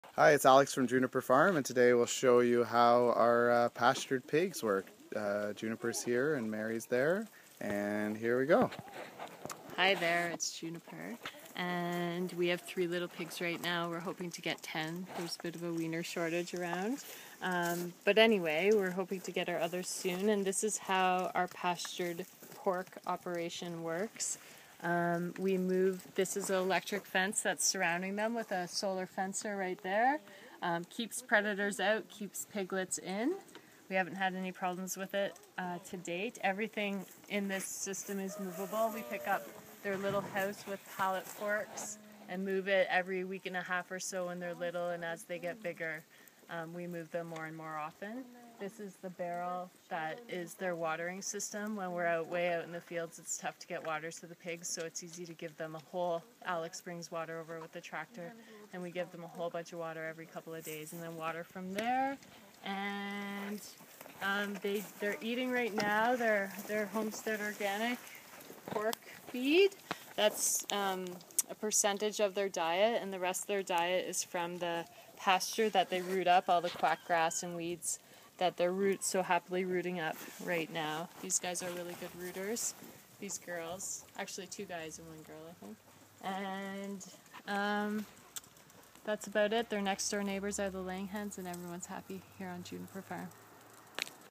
Juniper Farm pastured pigs.
With a moveable pen and fences we pasture our pigs on our organic vegetable farm.